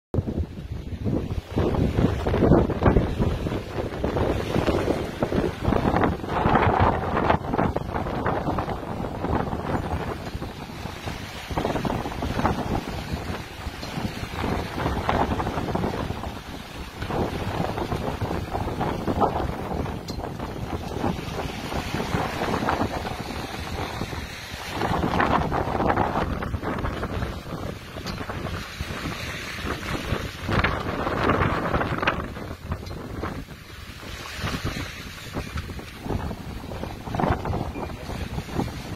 From La Rochelle to Hondarrabie ( North of Spain )
In 2019, we left Port des Minimes (La Rochelle) and crossed the Golf de Gascogne towards the North of Spain - Hondarrabie